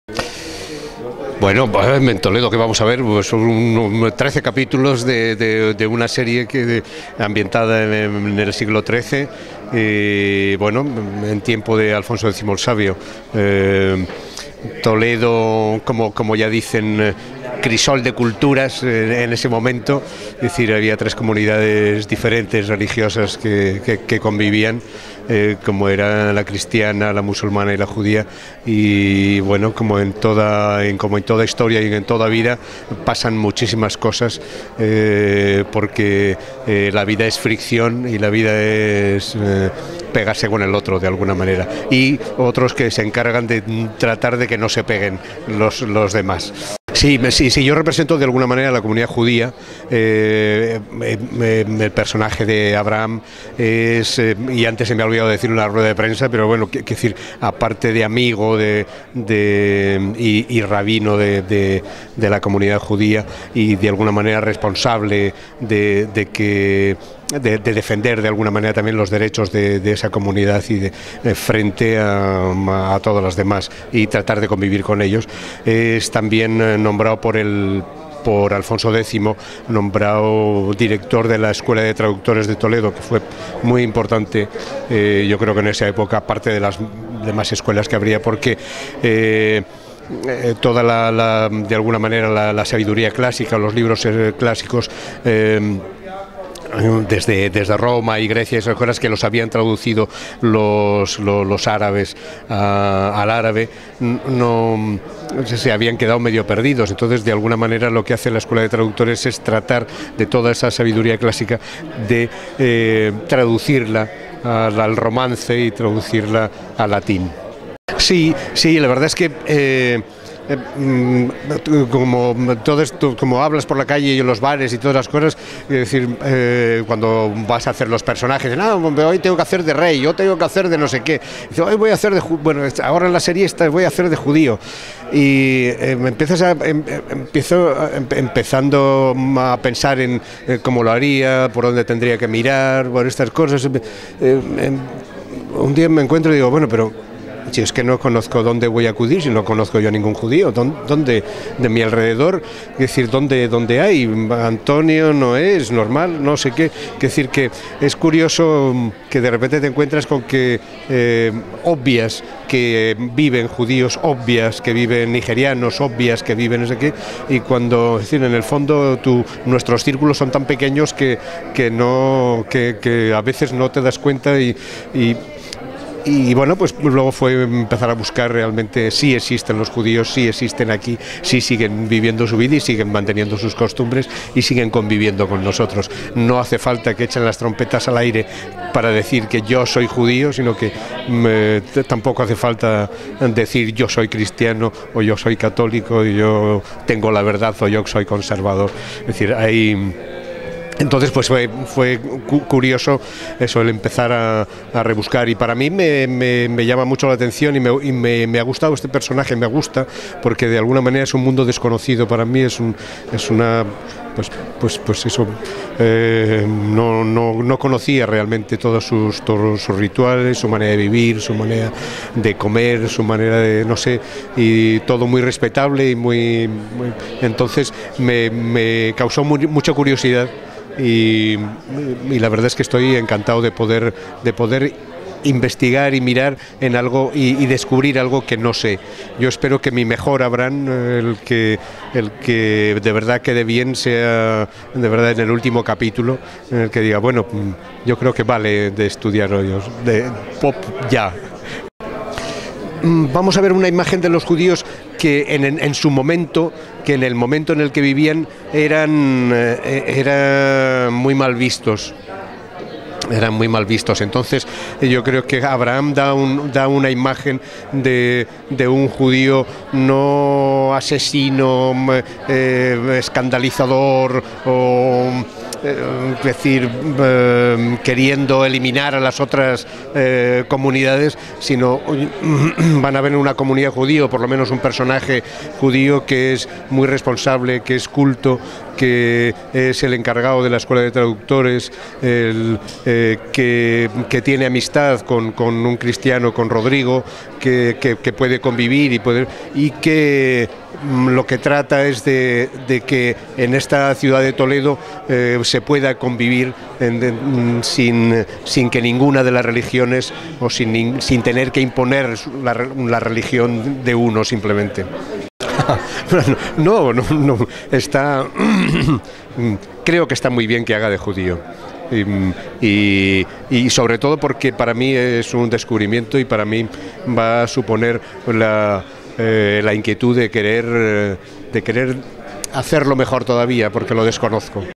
Álex Angulo: "Me gusta interpretar en 'Toledo' a Abraham porque no conocía el mundo judío"
Entrevista FormulaTV a Álex Angulo. El experimentado actor da vida al representante de la comunidad judía en 'Toledo', la nueva serie de Ida y Vuelta para Antena 3.